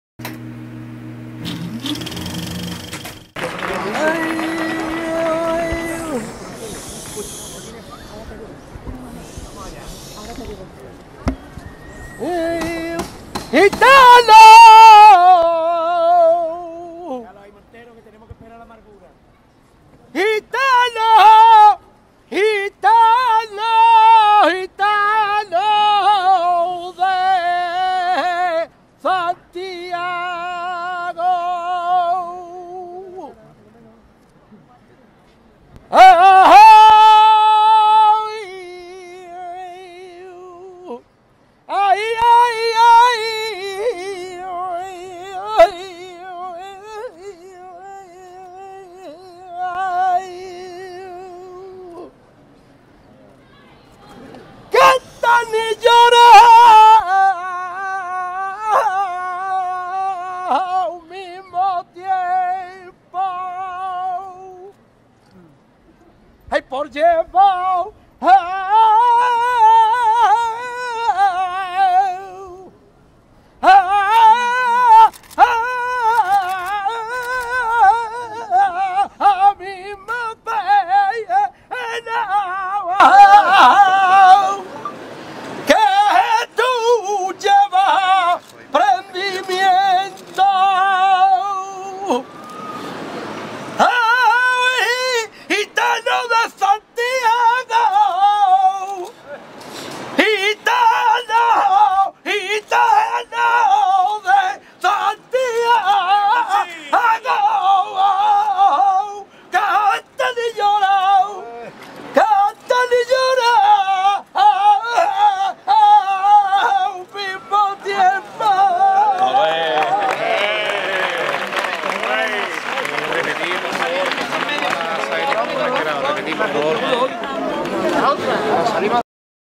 Joaquín el Zambo le dedica una saeta al Prendimiento
Saeta a 'El Prendi' de  Joaquin Fernandez Soto​ 'El Zambo' , ganador del  primer premio del concurso de saetas de la Peña 'Buena Gente' foto y video Juan Carlos Toro